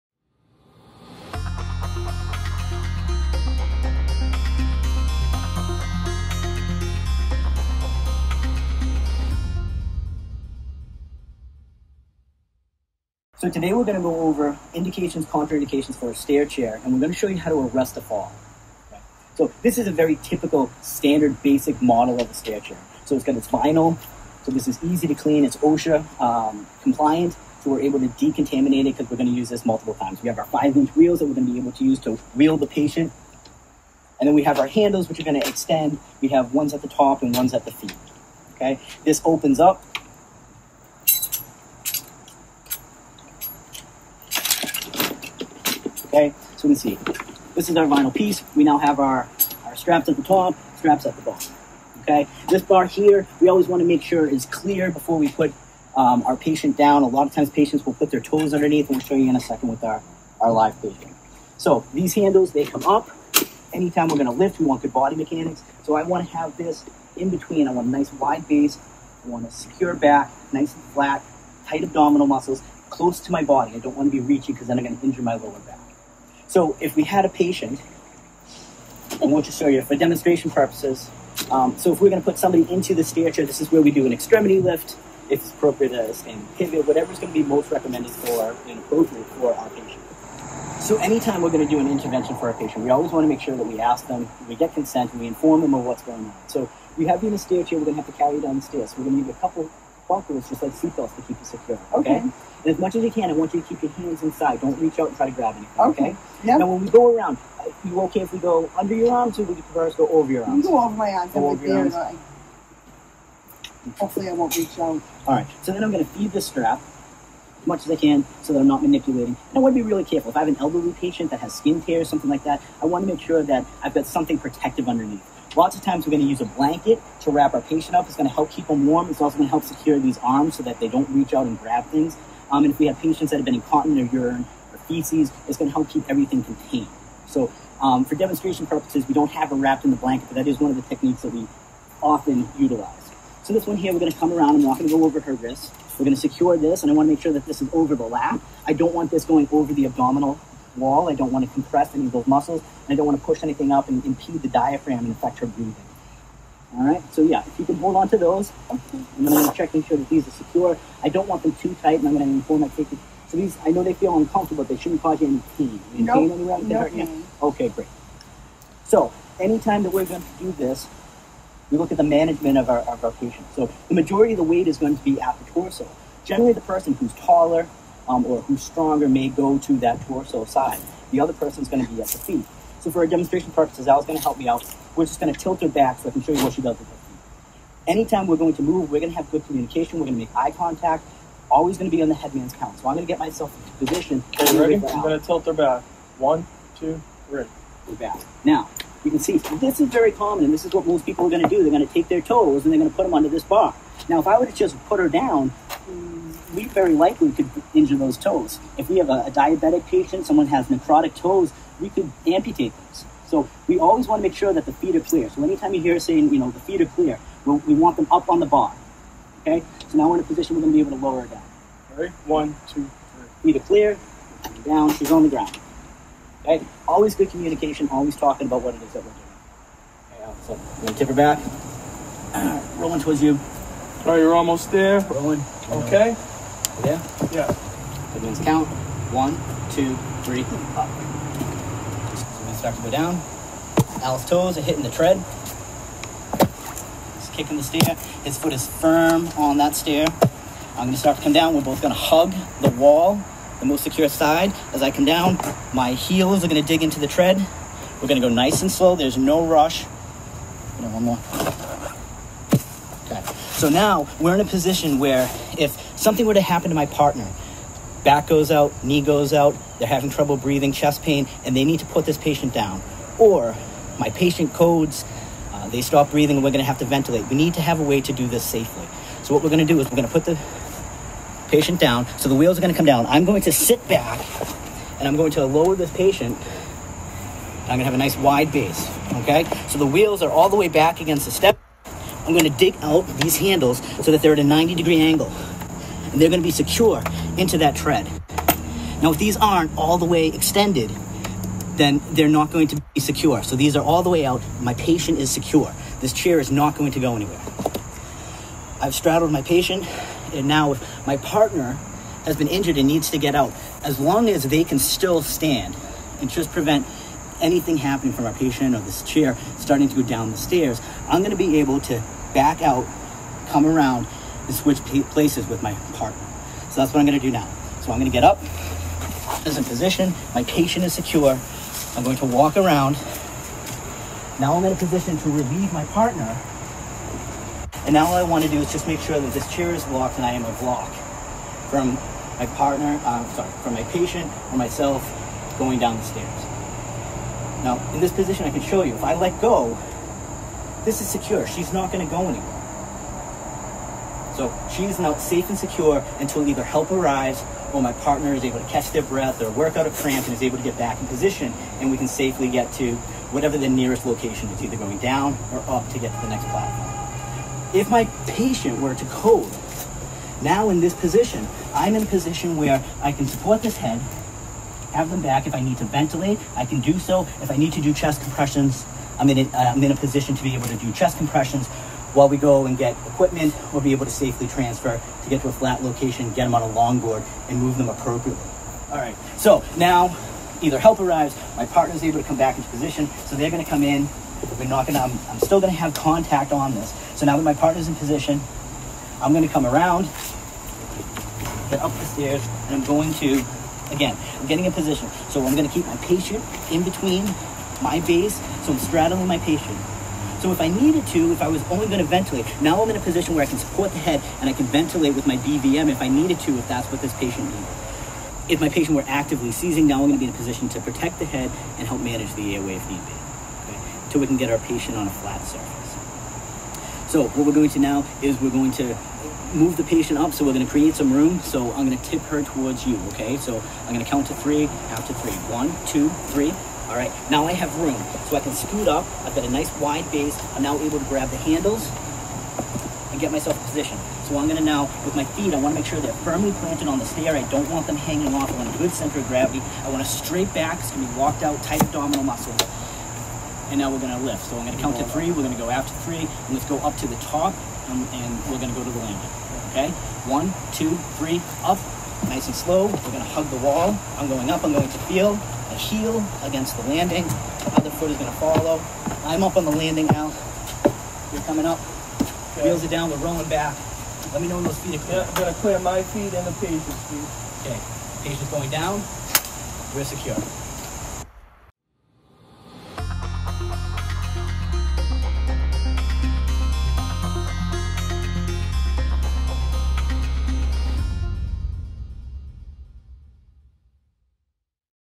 How to Arrest a Fall With a Stairchair
This video is a demonstration of how to use a stairchair and arrest a fall. A stairchair is used to carry patients up and down stairs safely. This video is intended for EMTs, paramedics, and firefighters.
STAIR CHAIRS: pieces of equipment designed to safely transport alert patients down stairs or steps and are an extremely useful piece of equipment in modern-day EMS. Patients should only be moved while secured to stair chair if they are alert and can maintain their own airway.